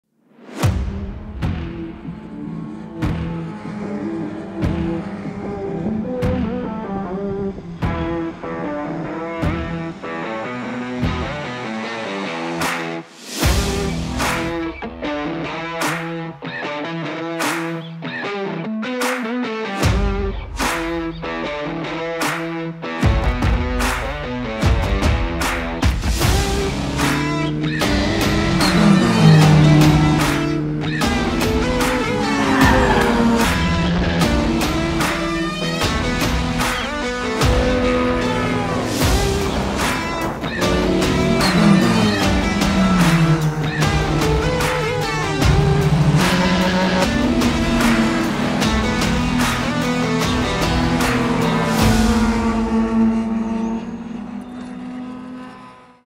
Autobahn Racing
Quick Summary of some great race days at Autobahn Country Club with the Lucas Oil School of Racing.